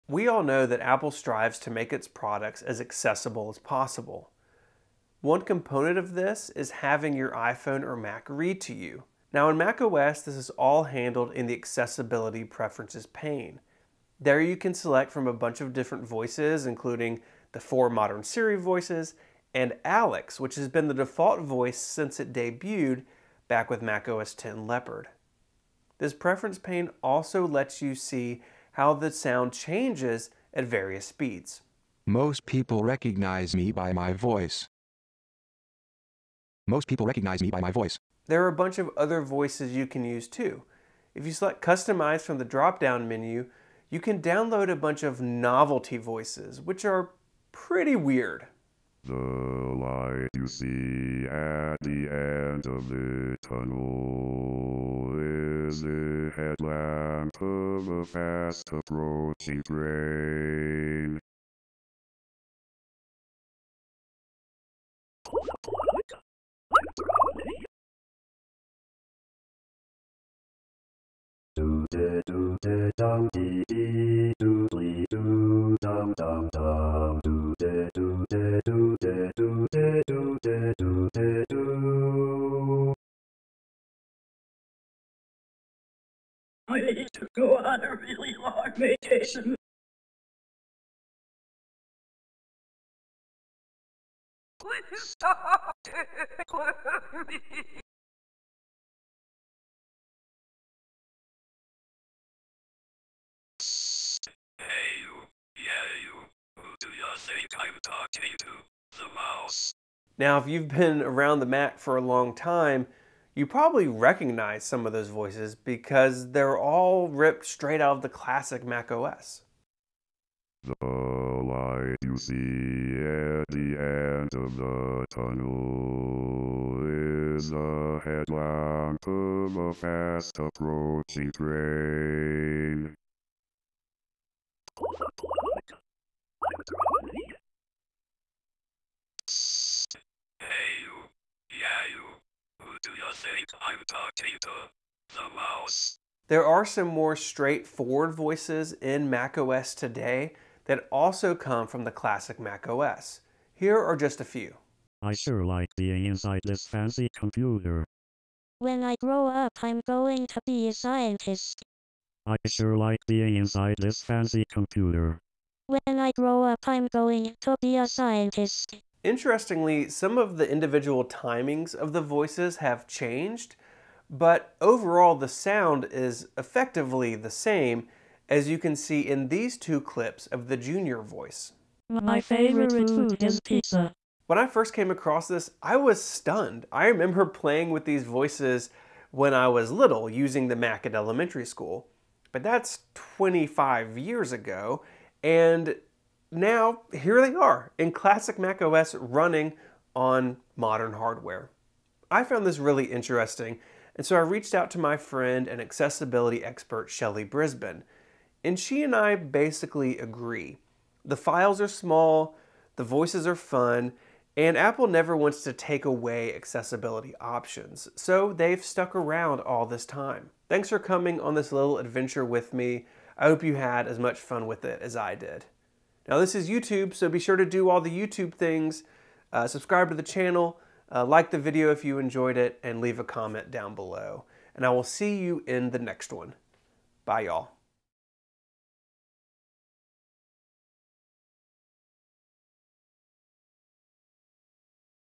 The Voices of macOS
The Mac has been able to speak since its introduction, and today the feature is core to the Accessibility story for macOS. However, some of the voices available today are straight from the classic Mac OS. In this video I compare some examples from Mac OS 9 with some from macOS Monterey.